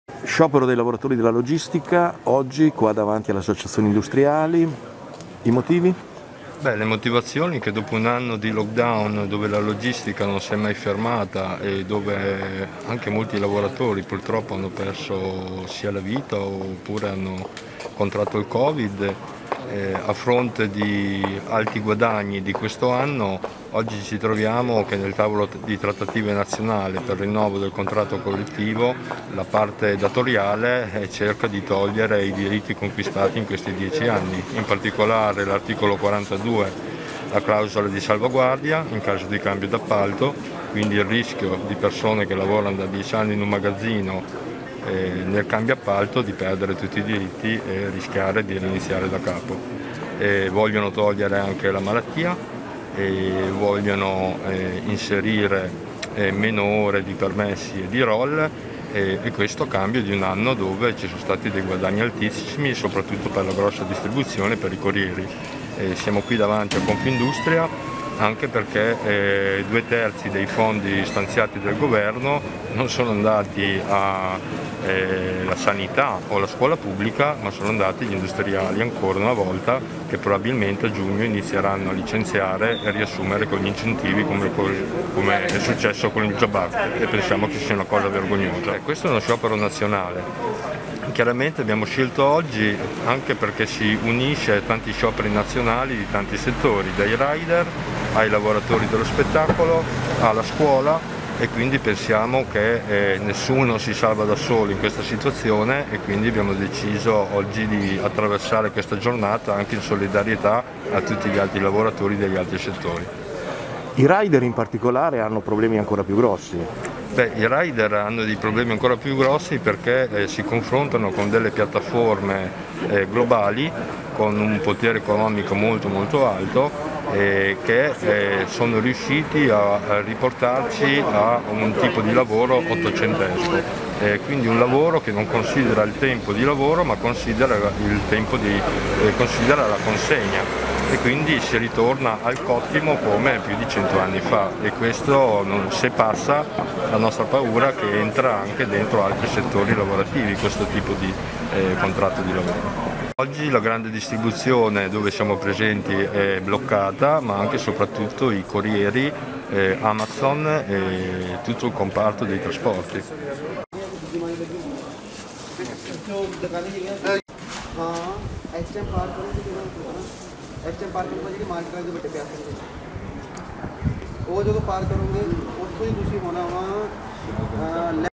Reggio Emilia, presidio dei lavoratori della logistica in via Toschi
In occasione dello sciopero nazionale dei lavoratori della logistica si &egrave; svolto un picchetto di protesta&nbsp;in via Toschi, sotto la sede di Confindustria. I lavoratori hanno denunciato "le provocazioni ai tavoli nazionali dalla parte datoriale che dopo 14 mesi che il contratto &egrave; scaduto e dopo un anno che i lavoratori della grande distribuzione non si sono mai fermate e l'aziende hanno aumentato i ricavi ora propongono di togliere le clausole di salvaguardia, la malattia pagata e i diritti sindacali". Video di Rossi Fotografi